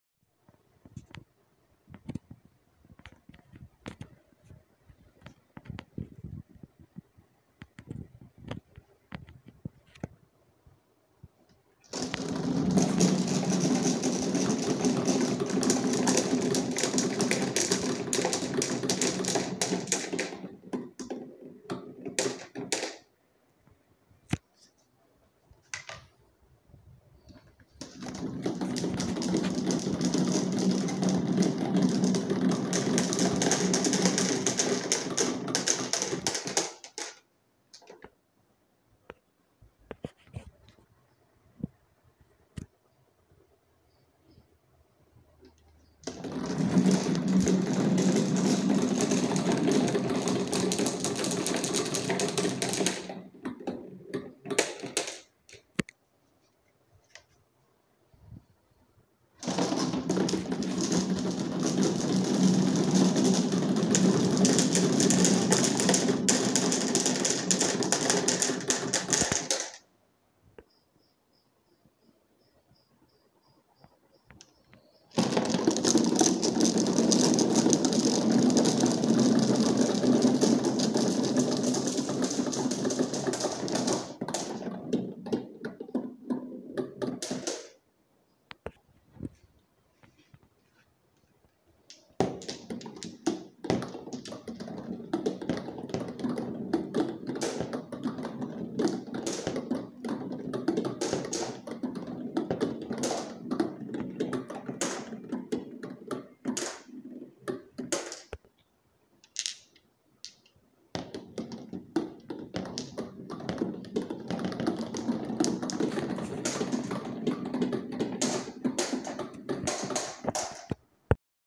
asmr marble run sounds effect and satisfying marbles motion (1)
asmr marble run sounds effect and satisfying marbles motion